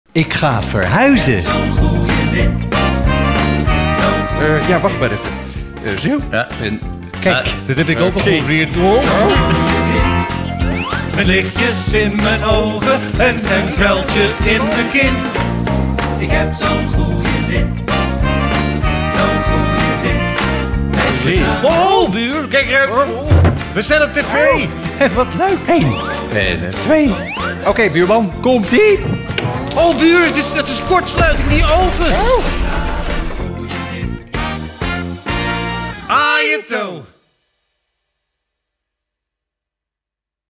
Zwolse theaters | Buurman & Buurman gaan verhuizen
zo 25 feb | theater de Spiegel
Buurman & Buurman 4+
Buurman & Buurman gaan verhuizen

Als Buurman & Buurman besluiten te verhuizen moet er natuurlijk van alles gebeuren. Meubels sjouwen, een nieuwe keuken installeren, de vloer schuren en er mag ook wel een nieuw behangetje aan de muur. Nog even de schilderijtjes netjes ophangen, en klaar! Als dat maar goed gaat… Hun lijfspreuk: ‘A JE TO!’ roept straks de hele zaal luidkeels mee.